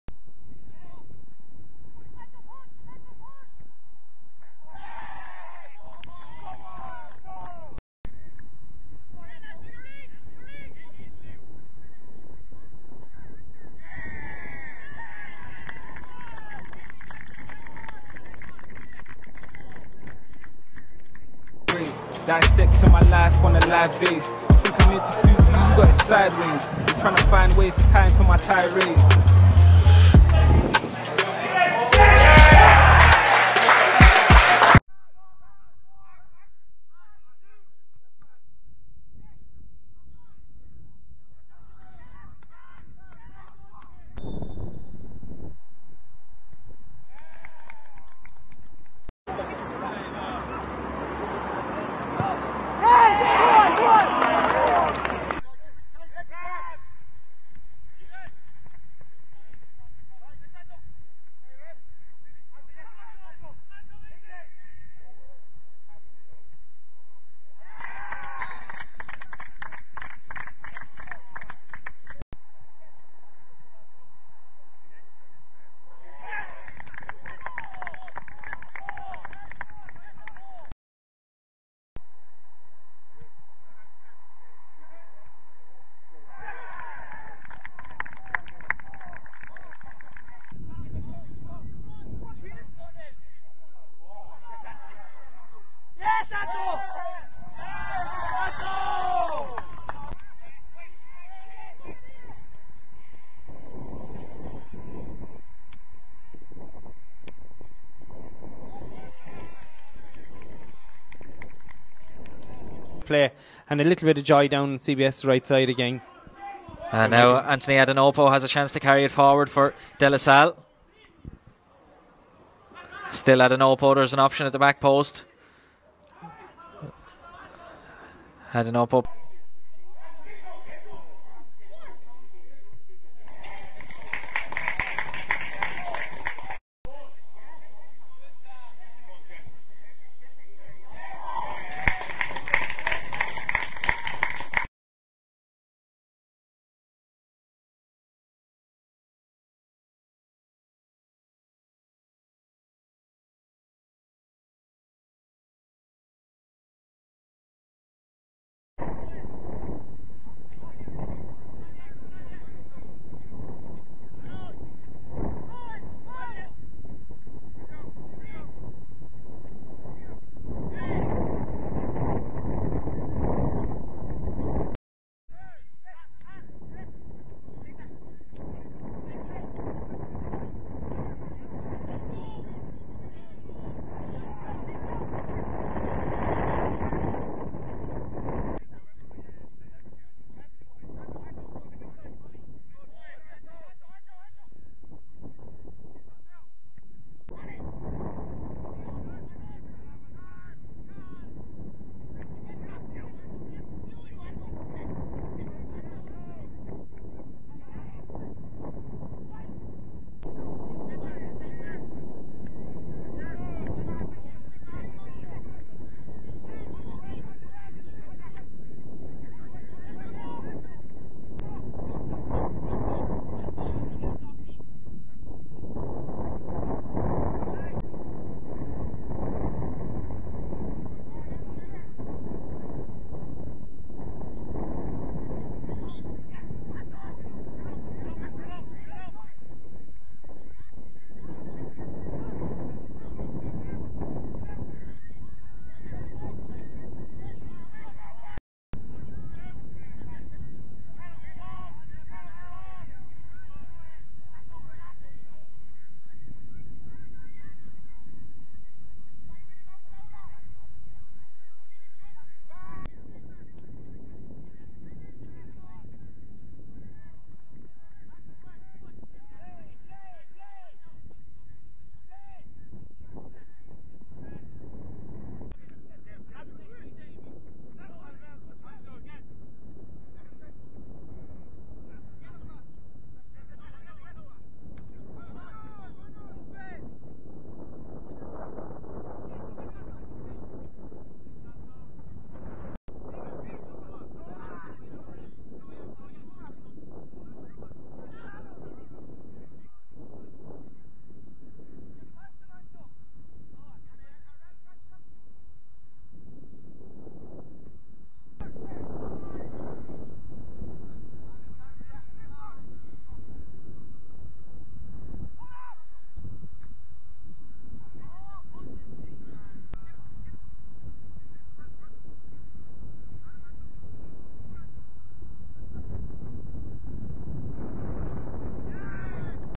Anthony Adenopo 1.85cm u18s , speed, Direct & powerful Irish Winger (EU) 2023 highlights,
Video from Eyeconic Sports Agents